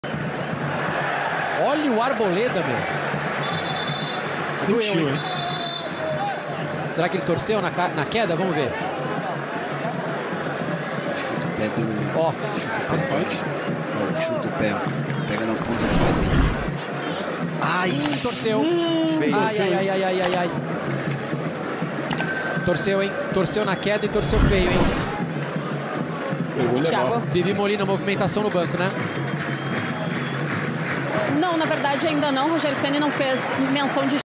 ¡TERRIBLE! 😱😭Robert Arboleda sufrió una grave lesión en los 8vos de final de la Copa de Brasil
¡TERRIBLE! 😱😭
Robert Arboleda sufrió una grave lesión en los 8vos de final de la Copa de Brasil, que lo podría dejar fuera de la Copa del Mundo de Qatar.